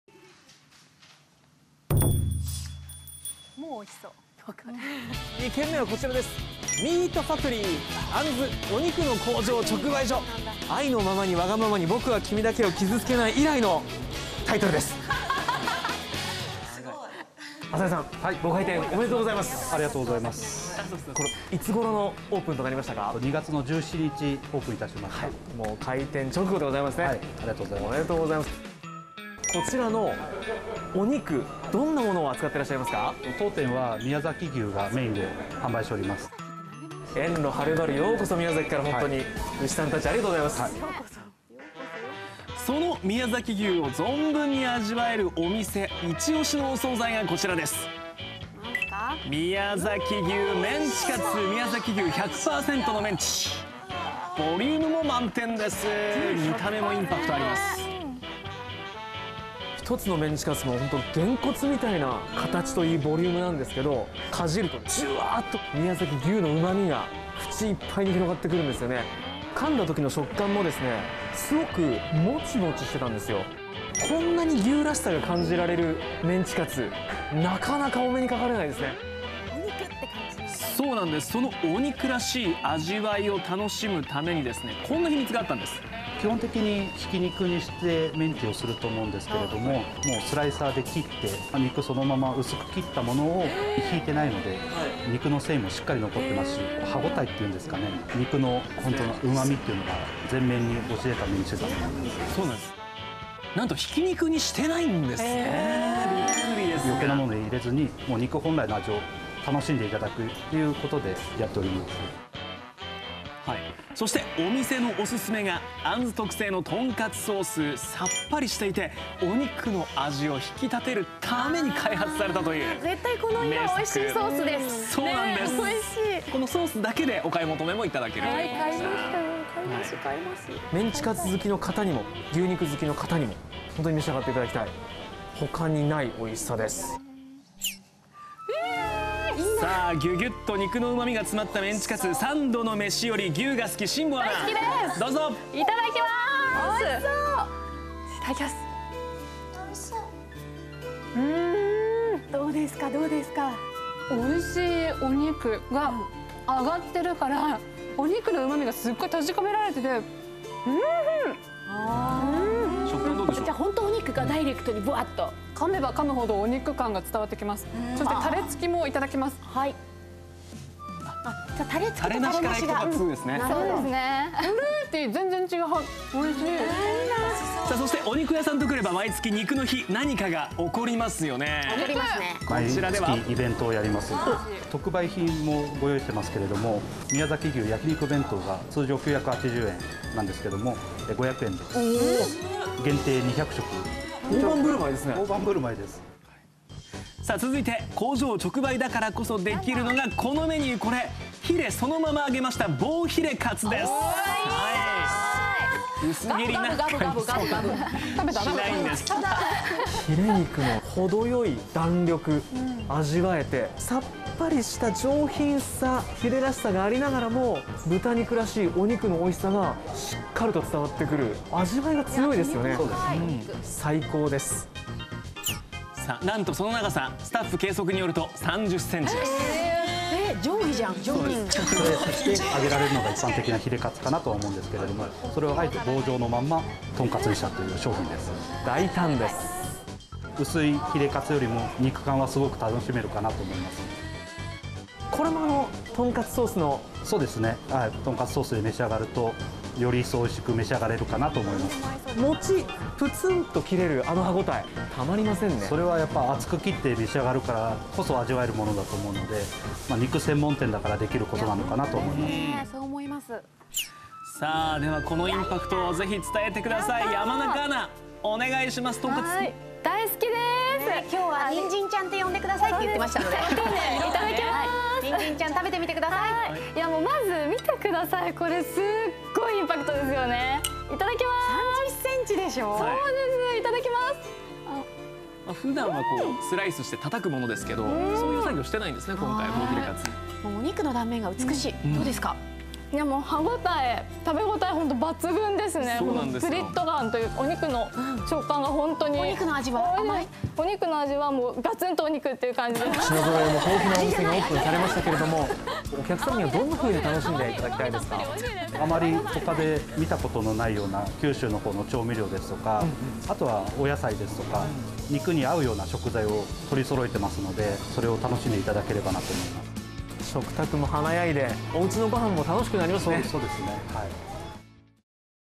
〈新潟おうちグルメ〉お肉の工場直売所 新潟市東区『あんず』 宮崎牛を一頭買い！お総菜も充実♪
自宅で本格料理を味わいたい！
 そんな方のために、新潟のおすすめ「お弁当・お総菜」販売店をご紹介します。

今回は新潟市東区の「あんず　お肉の工場直売所　新潟河渡店」。

九州が誇るブランド牛「宮崎牛」を扱う精肉店です。
一頭買いした宮崎牛をお店で解体する、まさに「お肉の工場」。

もちろん自慢の宮崎牛を使ったお総菜も充実しています。
人気は宮崎牛メンチカツ。
サクサクの衣とモチモチの宮崎牛の食感と、あふれる肉汁のうまみが楽しめます。
おいしさのヒミツはやっぱりお肉。
1センチほどの幅に刻んだ薄切り肉をタマネギと合わせて揚げることで、挽肉を使ったメンチより、お肉らしいうまみと食感が楽しめるんだとか。

豚のヒレをそのまま揚げた棒ヒレカツも人気。
自社製「とんかつソース」も肉のうまさを引き立てくれます！

毎月29日（肉の日）はイベントを実施。
お得に宮崎牛が楽しめます。

所在地 新潟市東区河渡新町1丁目7-9
営業 午前10時～午後7時

★2020年11月始動★
新潟のテレビ局NST新潟総合テレビのニュース配信専用チャンネルです。
新潟県内のニュースを配信します。

【NSTNewsタッチ】 　毎週月～金曜　18:09～19:00　放送中！